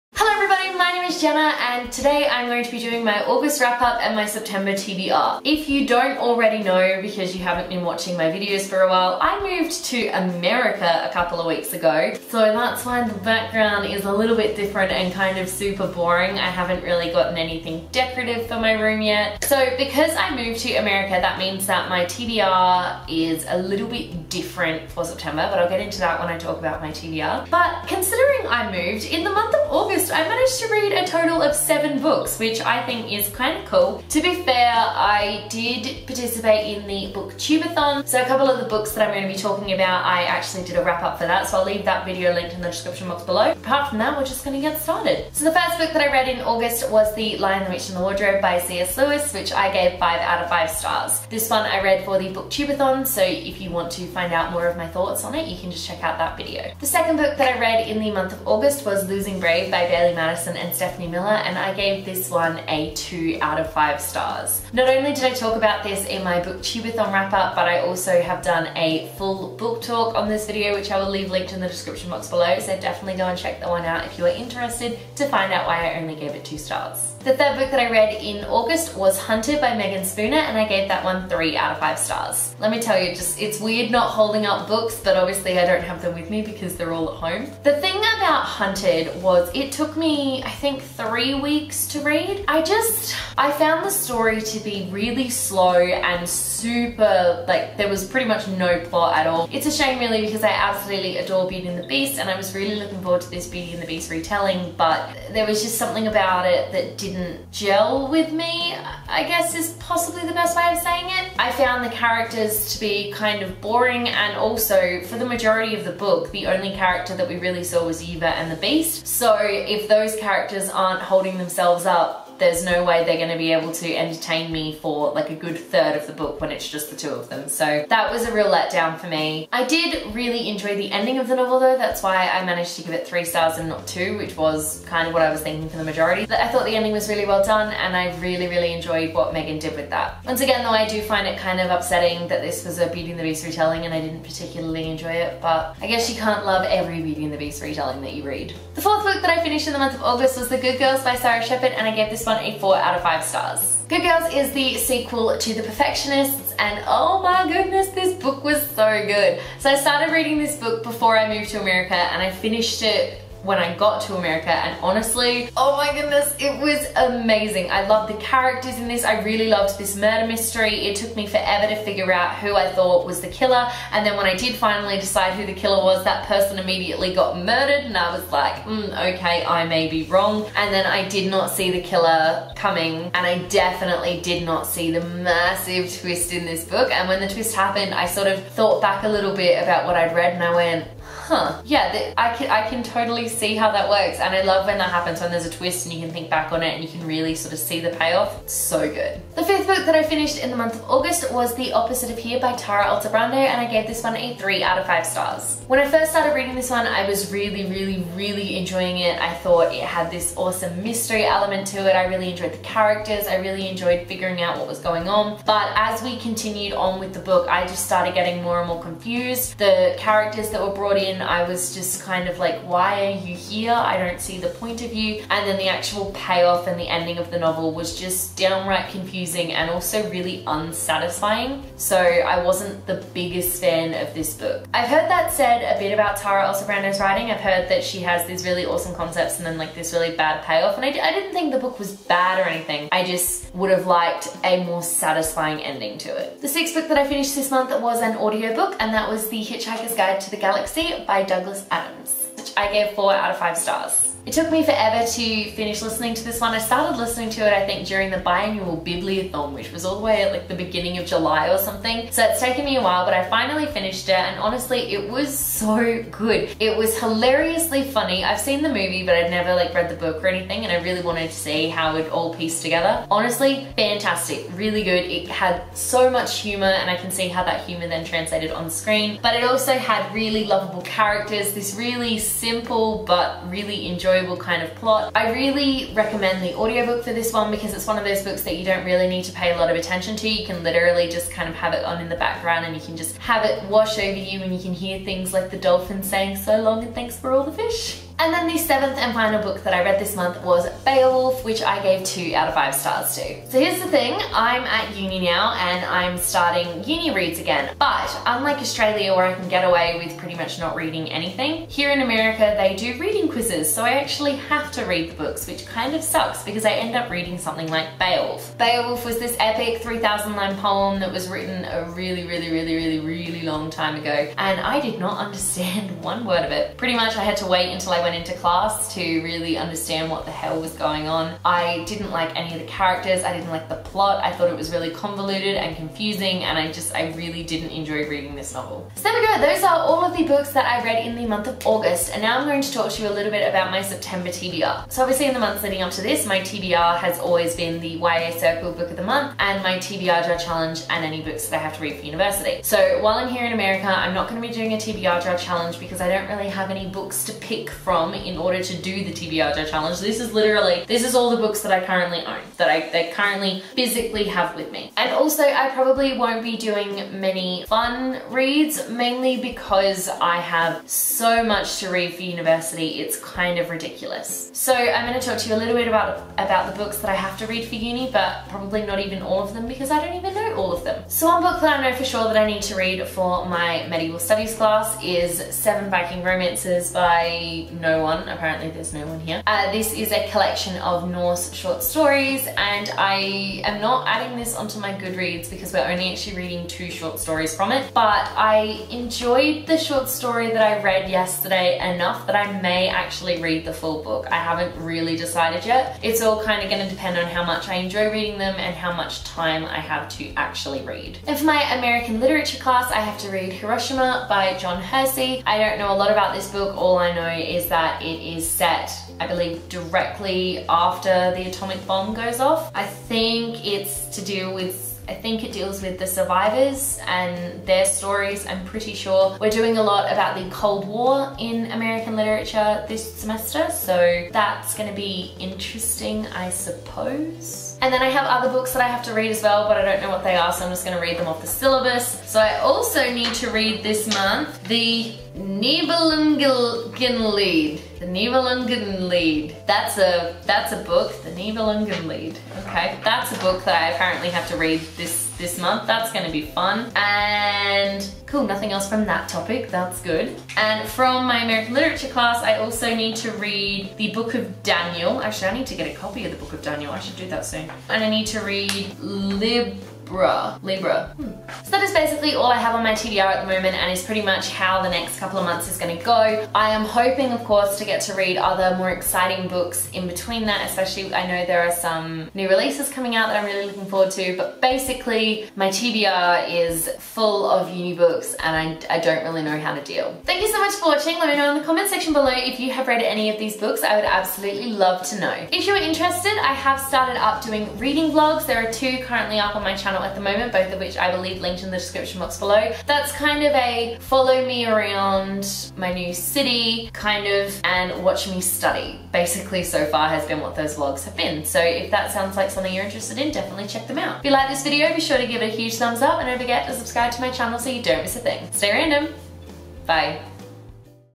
August Wrap-Up & September TBR | 2018
I read some books in August and I have to read a LOT of university books in September. Also HOW IS IT ALREADY SEPTEMBER? THIS IS NOT OKAY!

Stay random!
~ Jenna

GO BOOK SHOPPING!